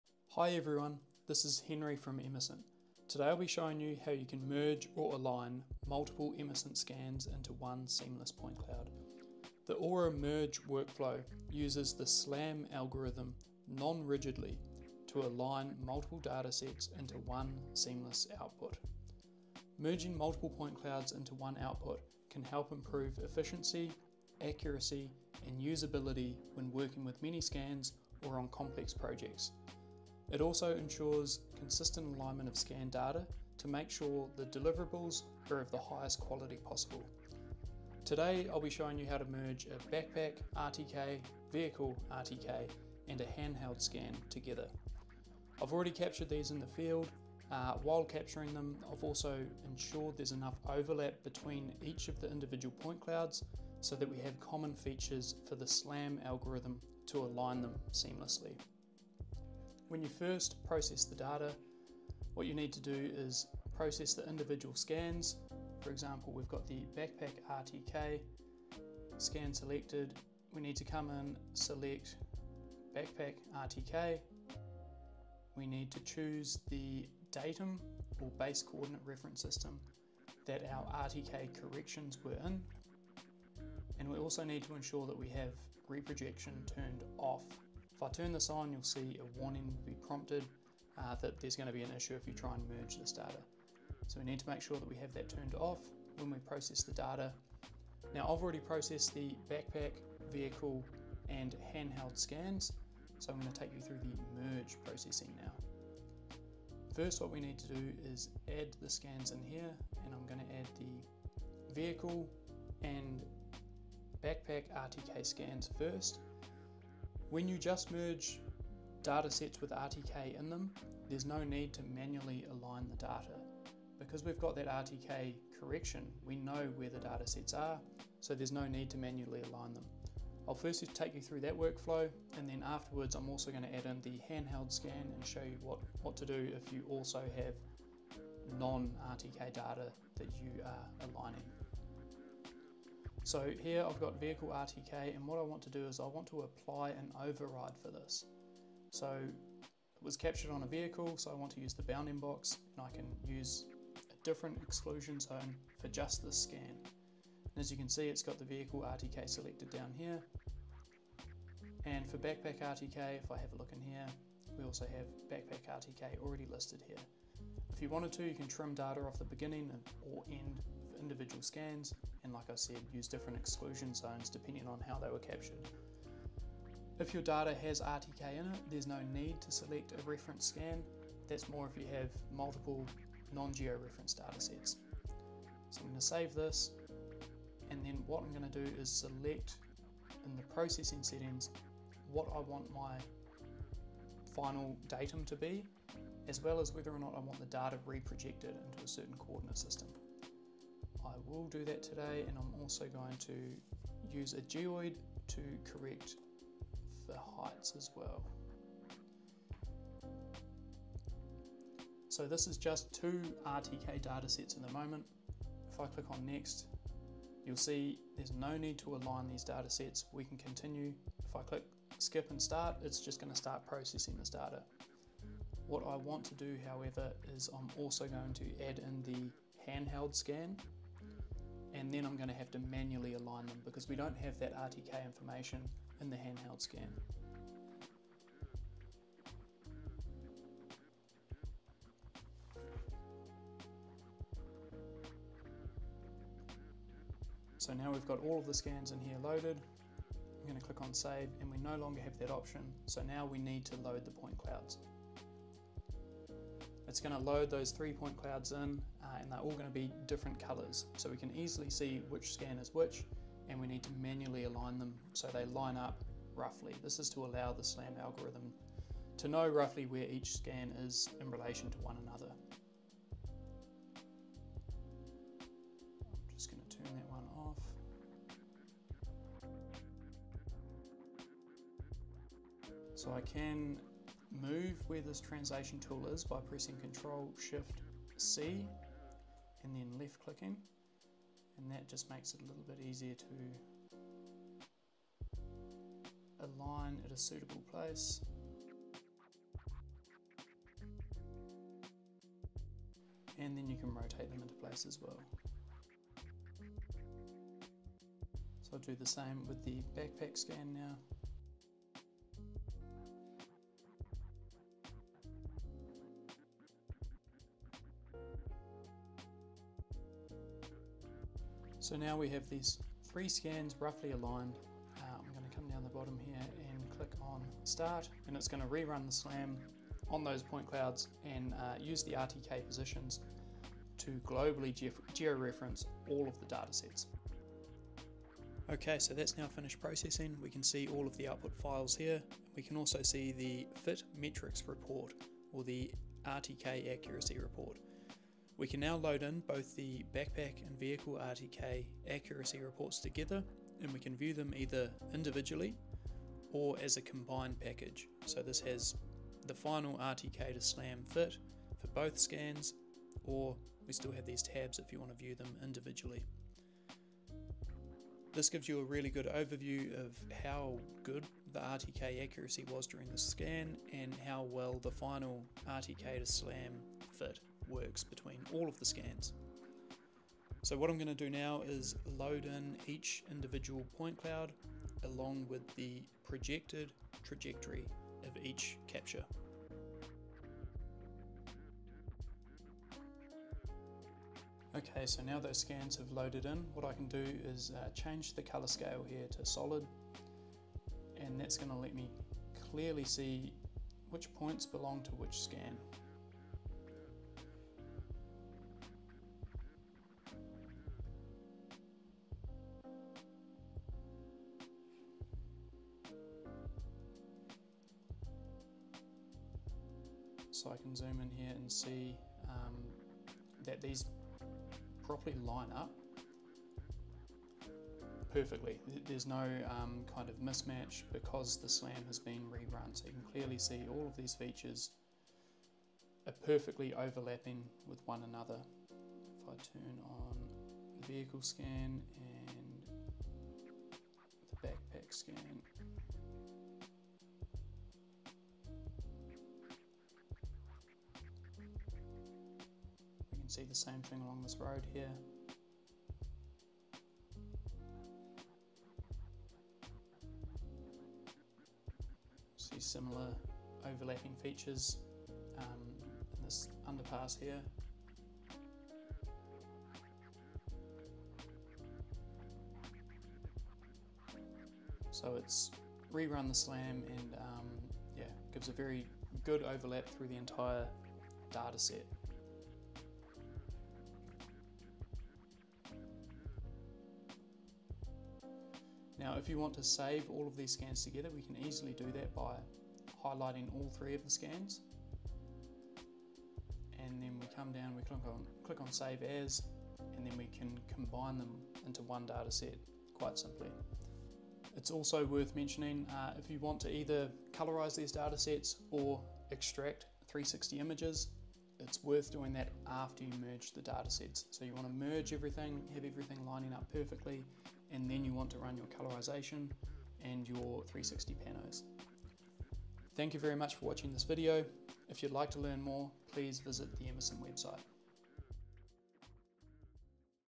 How to Merge Datasets
This video shows how to merge or align multiple Emesent scans into one seamless point cloud.

00:00 -  00:50 - Overview
01:00 - 01:36 - Process individual scans
01:37 - 04:09 - Merge processing workflow
04:10 - 05:34 - Manual alignment of scans
05:35 - 08:47 - Review merged datasets